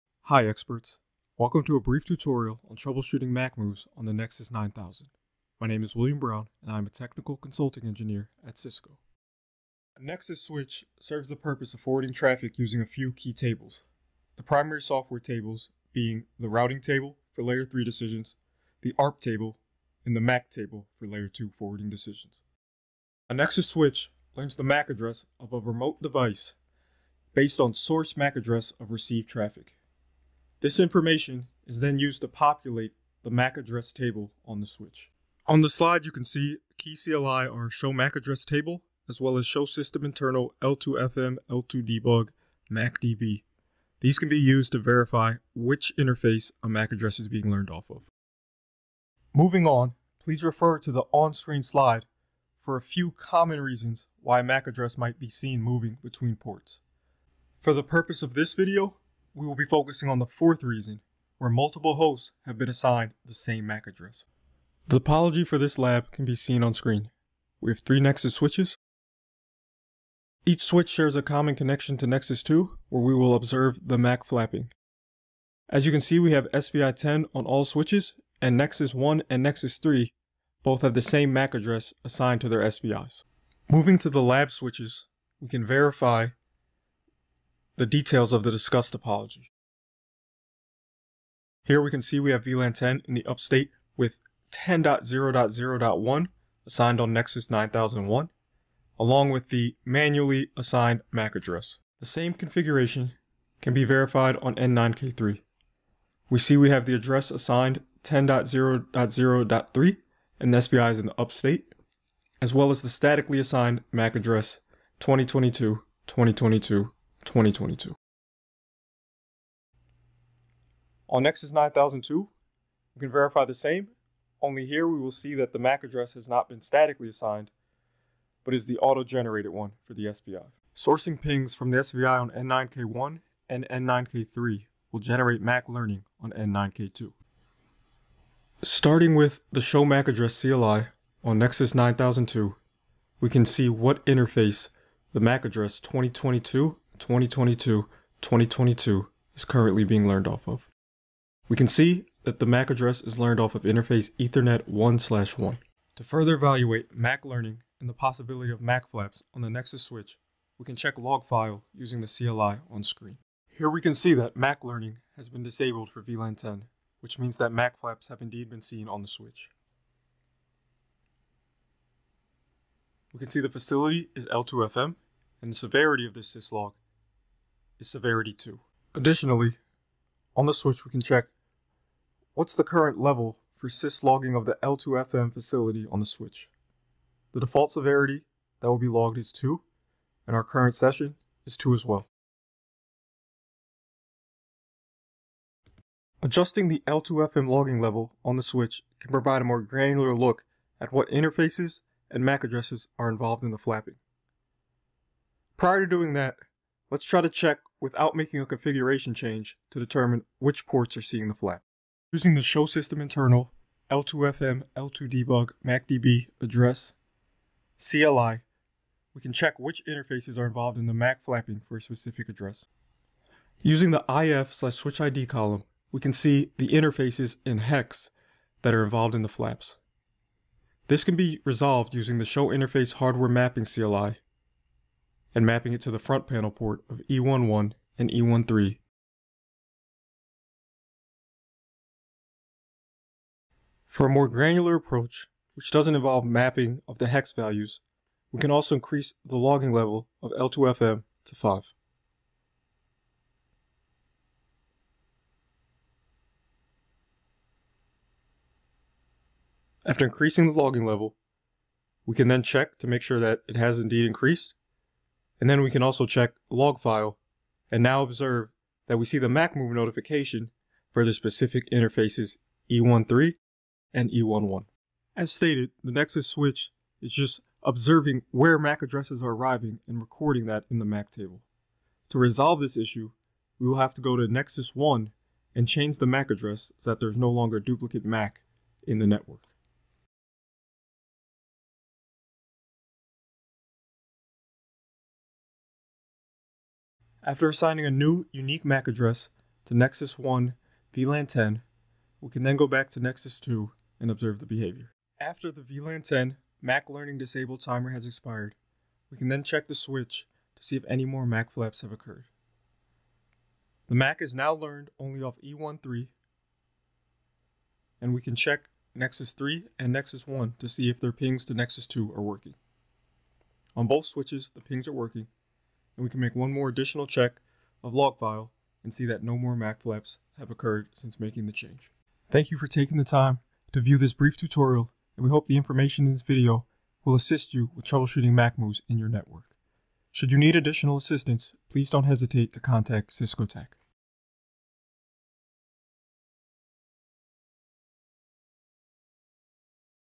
How to Troubleshoot MAC Address Flapping on Cisco Nexus 9000 Switches
In this Cisco Tech Talk, we show you how to identify and troubleshoot MAC address flapping issues on Cisco Nexus 9000 switches. MAC flapping occurs when a MAC address is seen moving rapidly between different switch ports—often causing network instability and degraded performance. This video outlines root causes, detection methods, and resolution strategies.

  What you’ll learn in this video:
  – What MAC address flapping is and why it occurs
  – How to detect flapping using show commands and logs
  – Common causes like port-channel misconfigurations or loops
  – How to resolve flapping and prevent future incidents

  Understanding and addressing MAC flapping ensures greater network stability and helps maintain optimal performance in your data center environment.